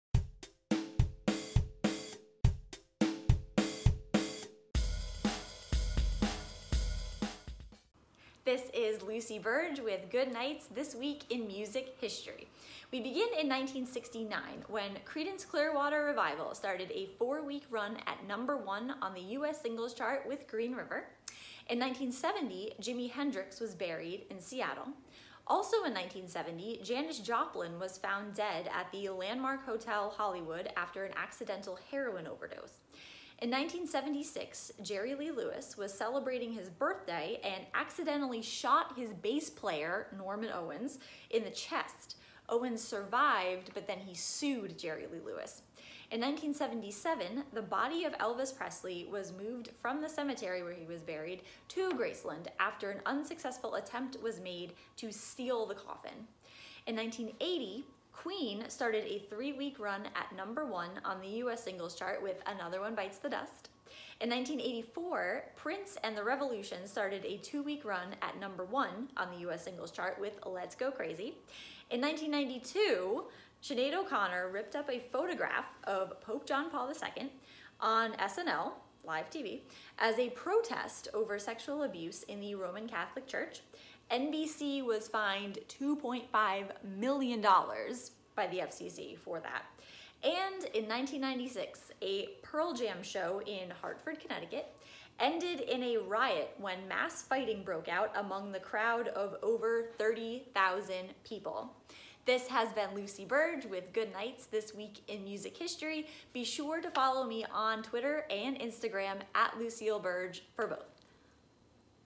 This Week in Music History 9/30
Join Lucy Burdge and travel through time as we take a look at significant music moments that happened on the week the show airs.
Catch Good Night every Monday at 9pm @